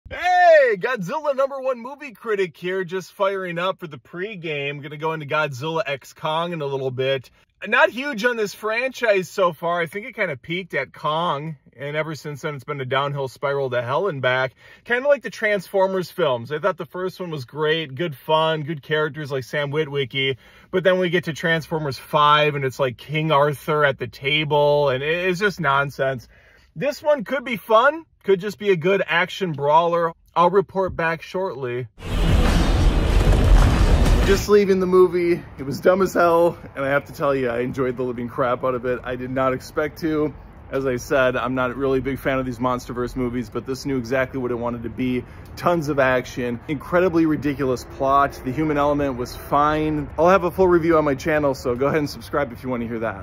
Godzilla x Kong: The New Empire Review Leaving The Theater
Godzilla x Kong: The New Empire Movie Review - I'm just leaving the theater so I wanted to give my initial reaction to seeing the new Godzilla X Kong movie.